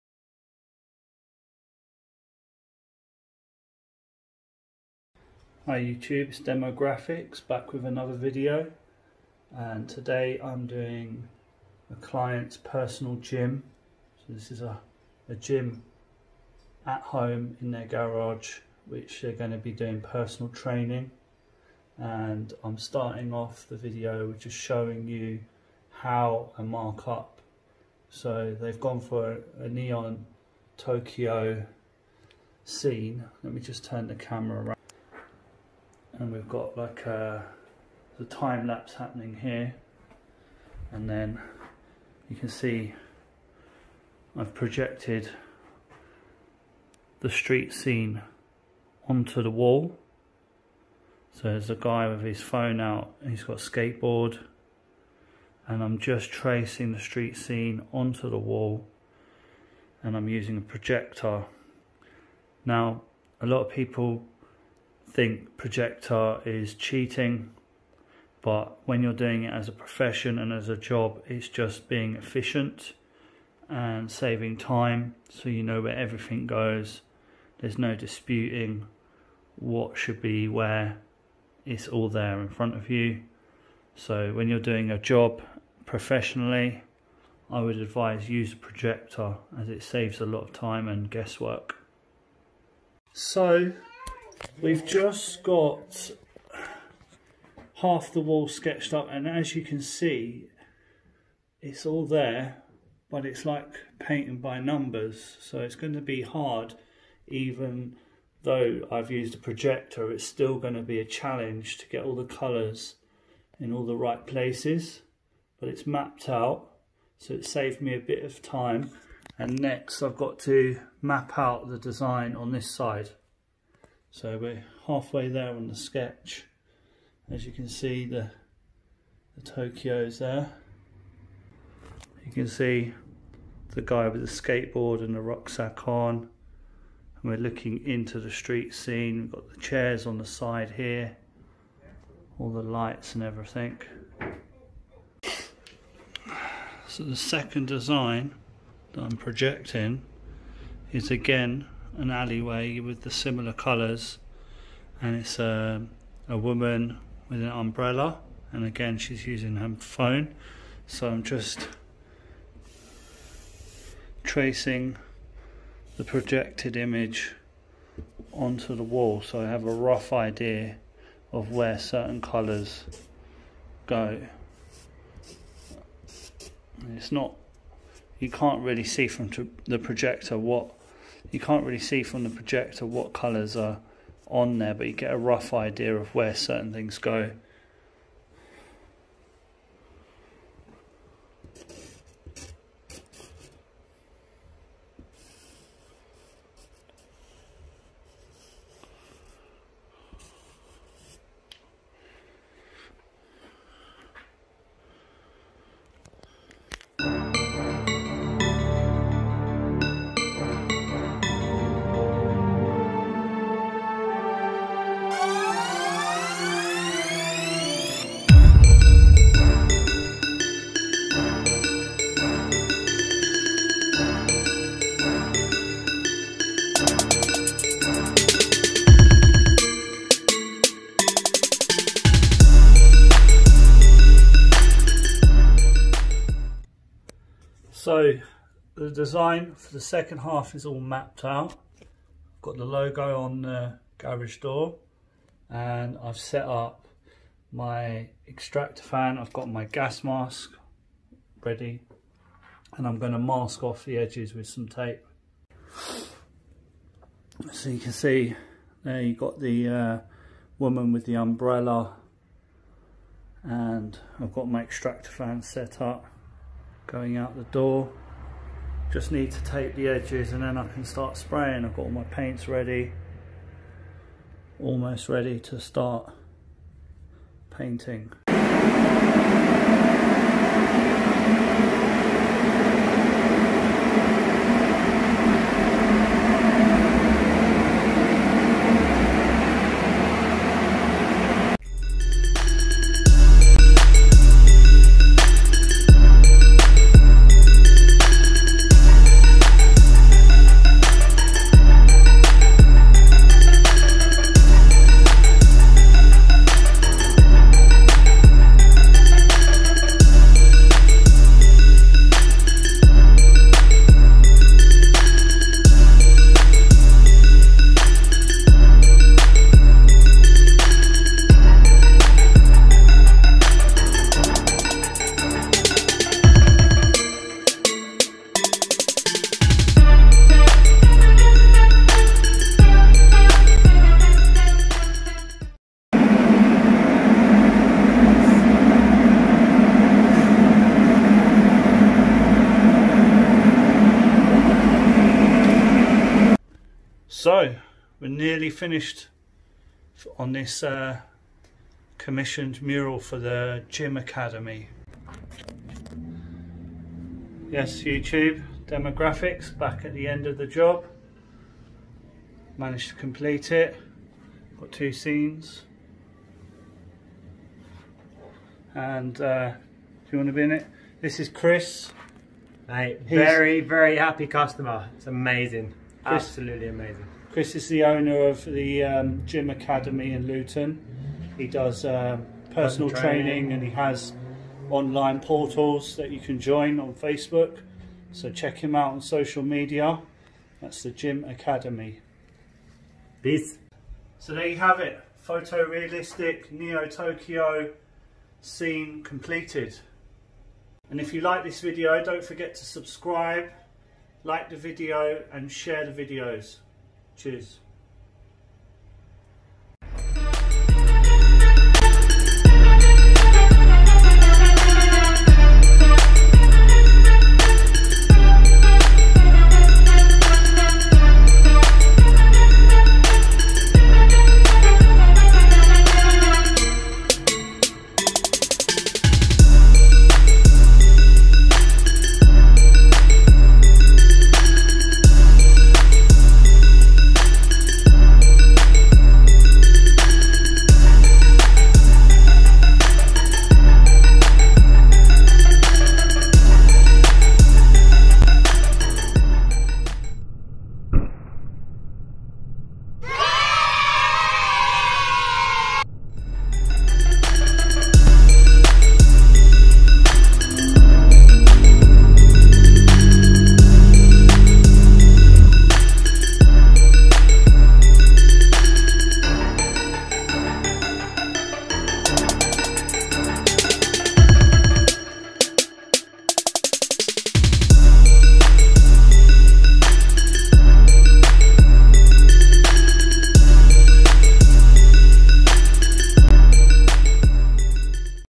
GRAFFITI - The Gym Academy Luton Mural
This mural was tough but I'm always up to the challenge, this took two days to paint but good things take time. Pleased with the sense of depth this mural creates its like you could jump into the neon Tokyo scene. Whats your favourite part of the image? what are your eyes most drawn too?
GOPRO COMMUNITY
DJI SPARK DRONE
URBEX LIFE
THETA S 360 CAMERA

You can also check out the SOCIAL MEDIA PAGES BELOW.